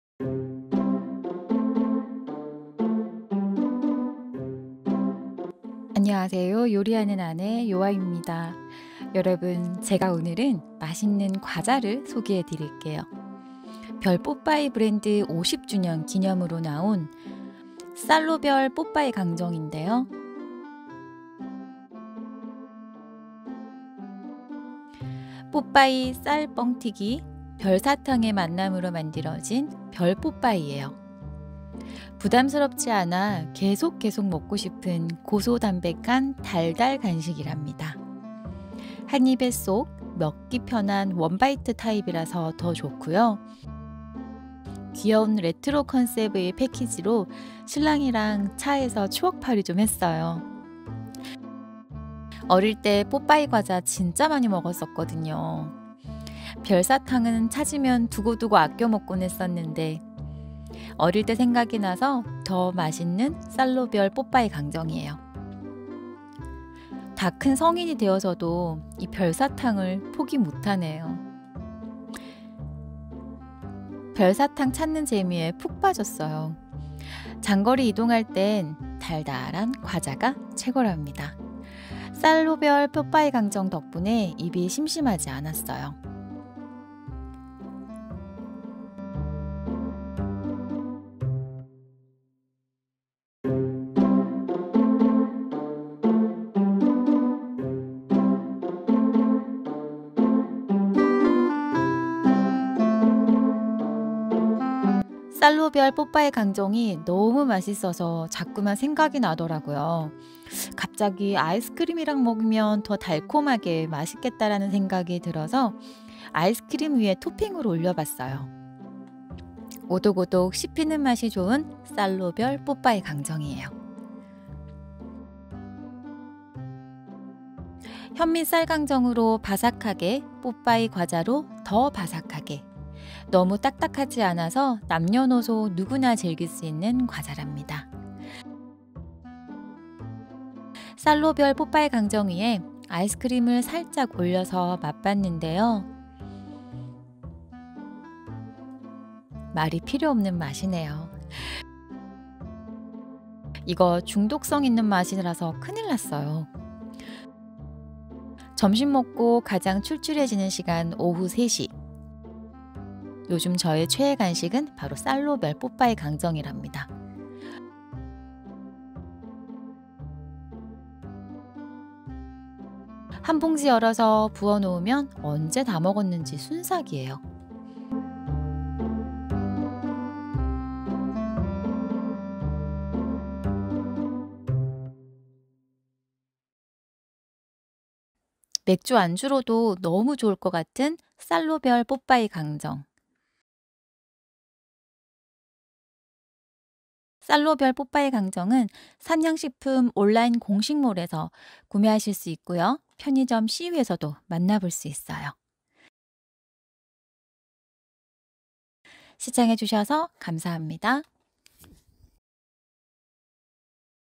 추억 돋는 맛있는 신상과자 쌀로별뽀빠이강정 달달하면서 바삭한 식감이 굿!
안녕하세요? 요리하는 아내 요아입니다.
오늘은 멈출 수 없는 고소 담백 달달한 과자! 쌀로별뽀빠이강정을 소개할게요
뽀빠이 과자, 쌀뻥튀기, 별사탕!!  3가지 재미있는 식감의 스낵인데요
한 입에 쏘옥, 먹기 편한 원바이트 타입이라 더 좋더라고요
별뽀빠이 브랜드 50주년 기념으로 새로운 형태와 식감의 신제품을
출시 했는데요 영상으로 함께 해주세요!!

참!! 구입은 삼양식품 온라인 공식몰 (네이버 스토어팜) 이나
편의점 CU에서도 만나볼 수 있어요